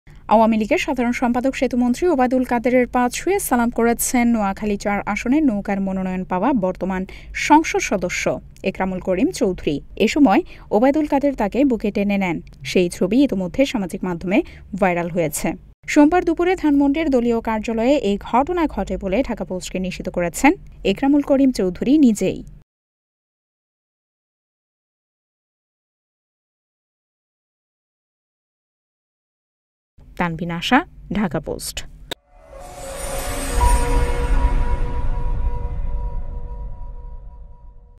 পা ছুঁয়ে সালাম একরামের, বুকে টেনে নিলেন ওবায়দুল কাদের | Awami League | Latest News | Dhaka Post
আওয়ামী লীগের সাধারণ সেতুমন্ত্রী ওবায়দুল কাদেরের পা ছুঁয়ে সালাম করেছেন নোয়াখালী-৪ (সদর-সুবর্ণচর) আসনের নৌকার মনোনয়ন পাওয়া বর্তমান সংসদ সদস্য একরামুল করিম চৌধুরী।.